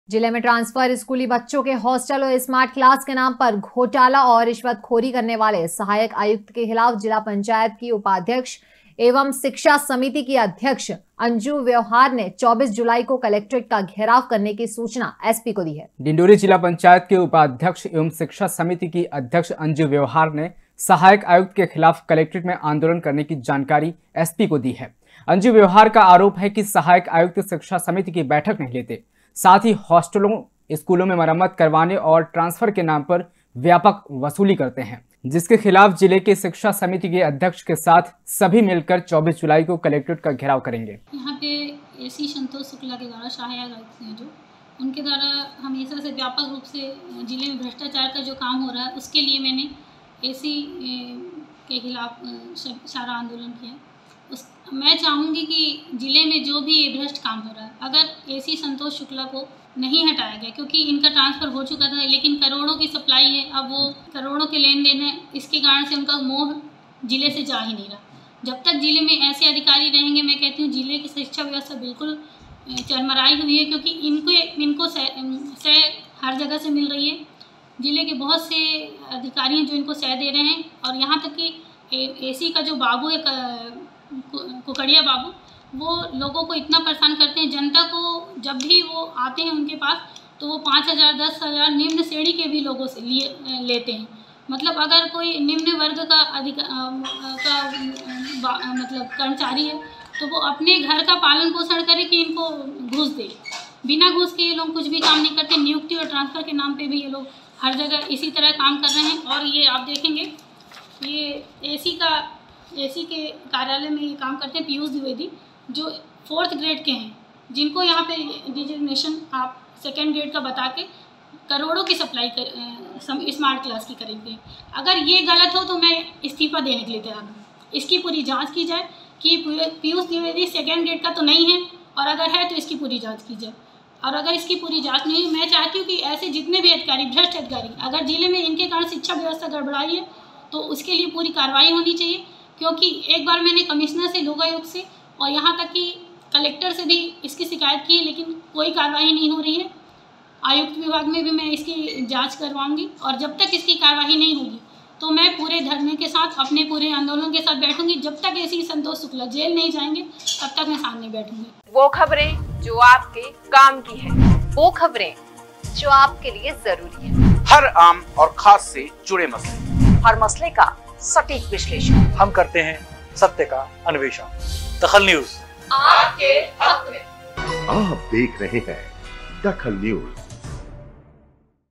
DAKHAL NEWS सहायक आयुक्त पर घोटाले के आरोप
सहायक आयुक्त पर घोटाले के आरोप
शिक्षा समिति के सदस्य देंगे धरना